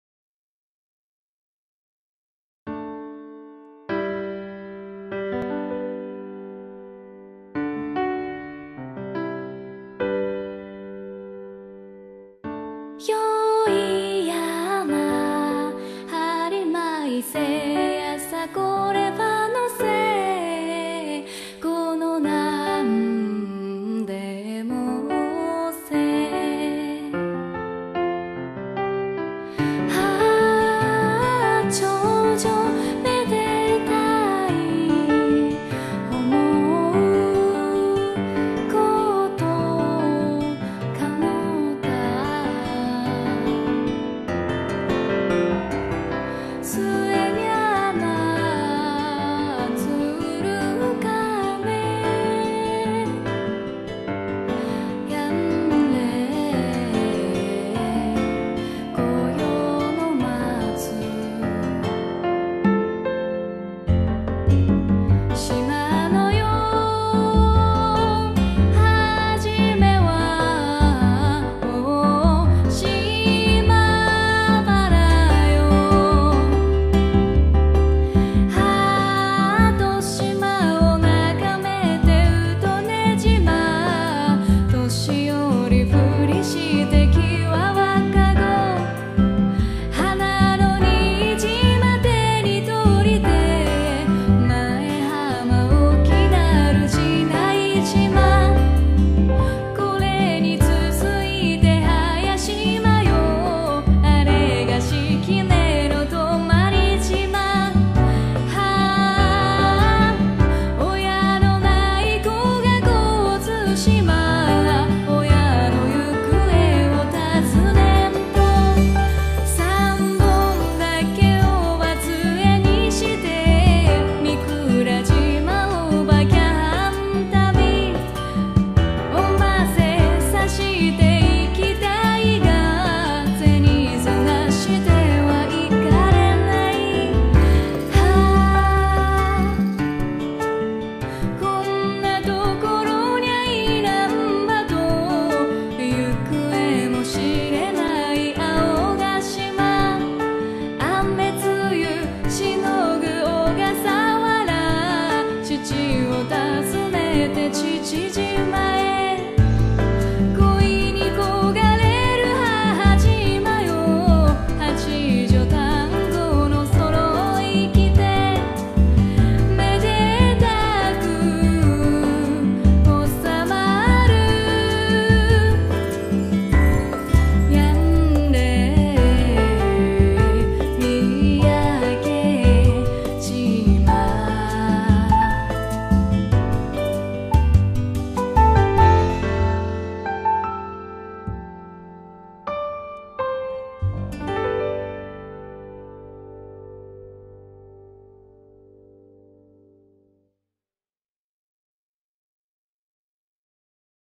神津島「神津島へ行こう（2013 11）」
「神津島へ行こう」を今回は、秋の風景（2013.11）で作ってみました（広報）。
今後も島の風景等をアップロードしていきますので、是非、登録してみてください！！